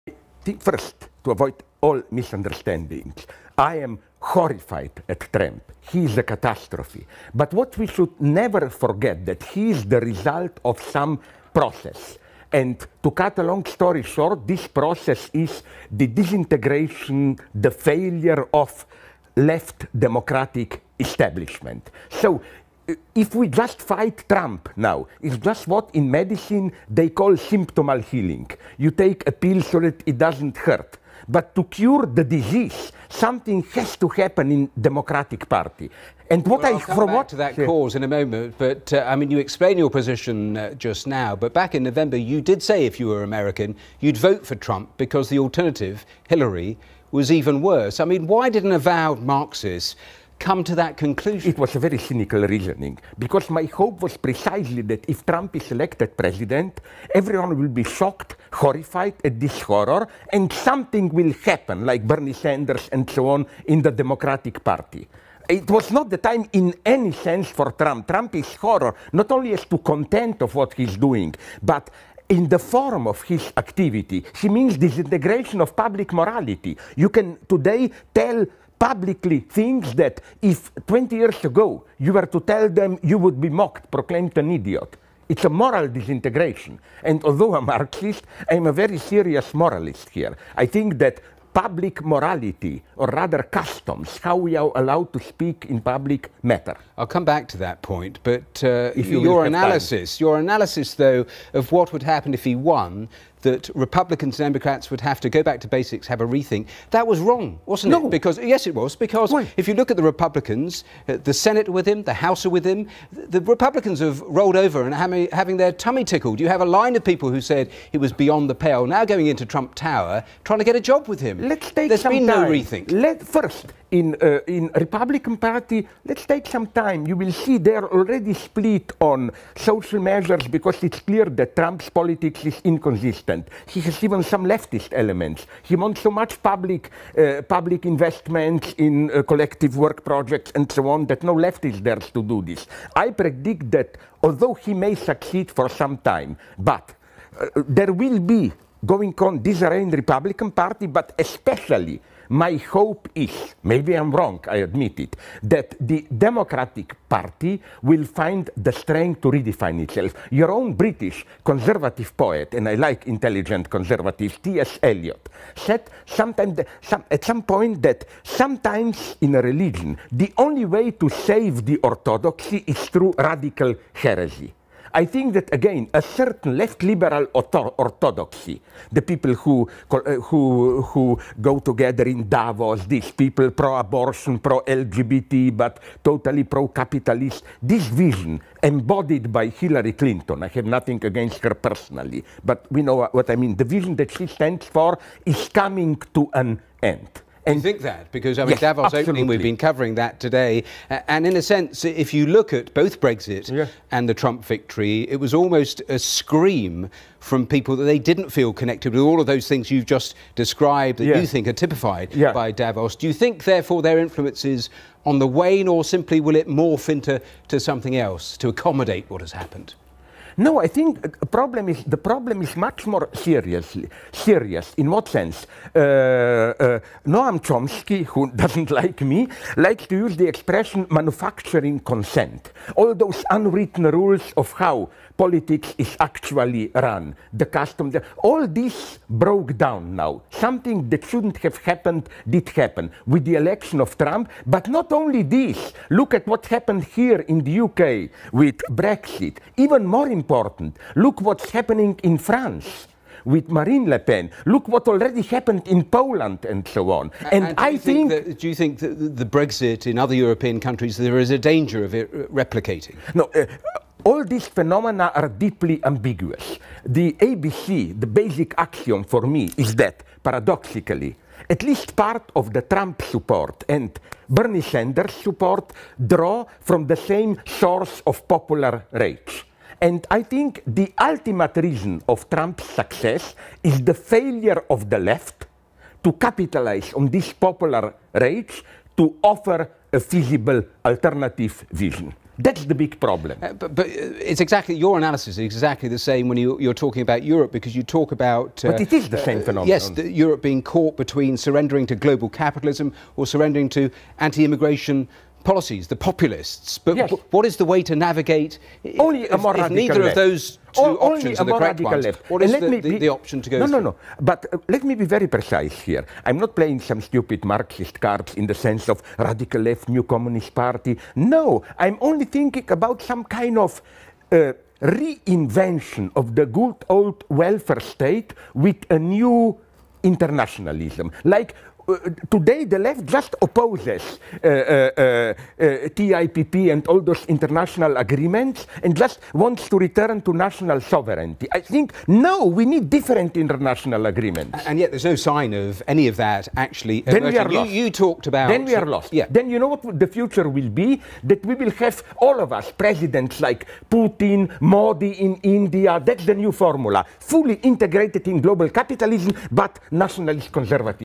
Slavoj Žižek Explains Why He Chose Trump (Jan. 2017)
Slavoj Žižek explains why back in November he said he would vote for Trump and gives us a glimpse of the near future if nothing is done to stop him (and other like him - Putin, Erdogan, Modi, etc.)

For UK viewers
And for German viewers